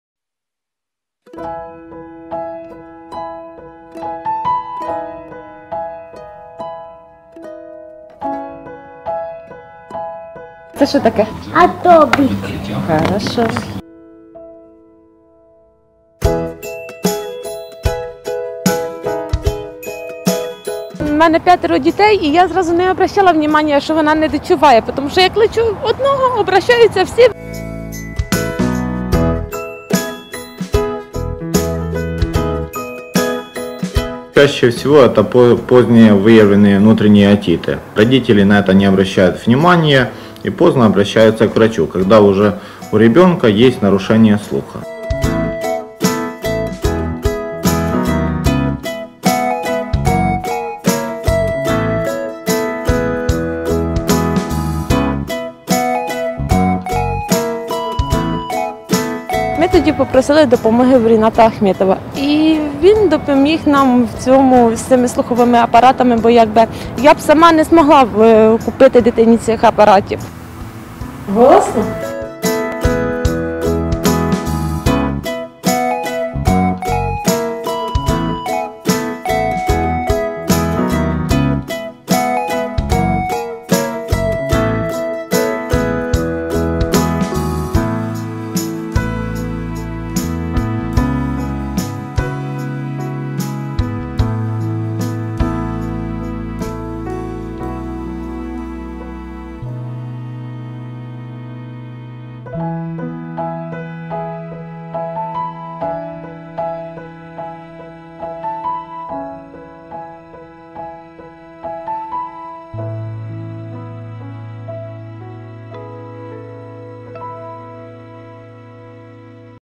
200 хлопчиків та дівчаток отримали слухові апарати
Завдяки проєкту «Рінат Ахметов – Дітям. Тепер я чую» понад 200 хлопчиків та дівчаток отримали сучасні слухові апарати. Високочутливі протези налагоджуються під індивідуальні особливості дітей, що дозволяє їм чути та повноцінно розвиватися. Однією із учасниць проєкту стала семирічна Владислава Река.